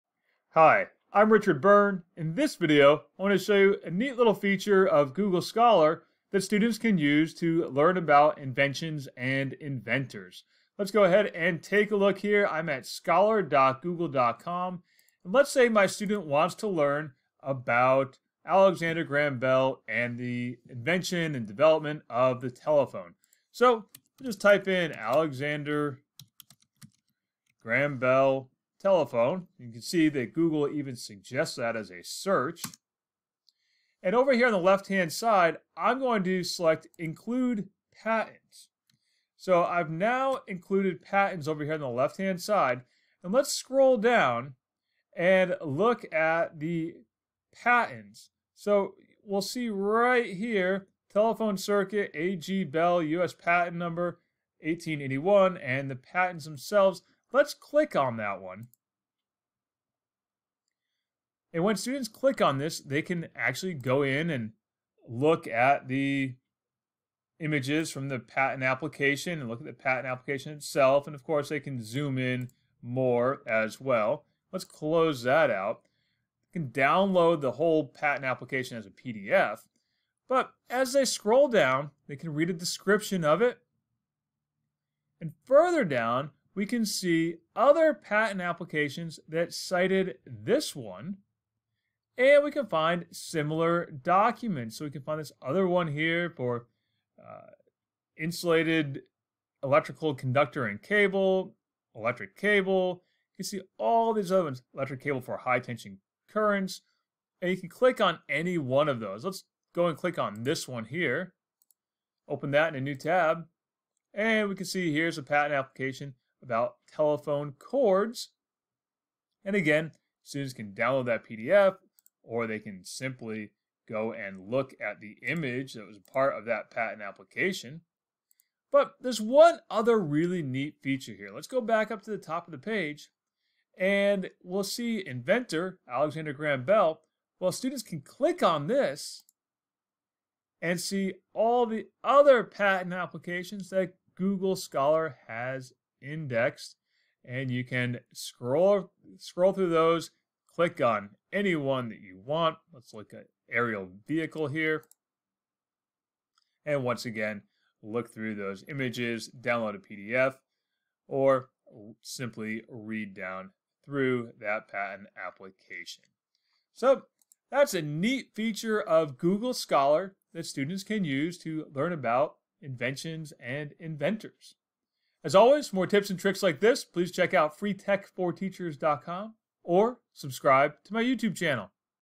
Use Google Scholar to Learn About Inventors and Inventions
Google Scholar includes an option to find patent filings and read them online. Using that feature is a neat way to learn about inventions and inventors.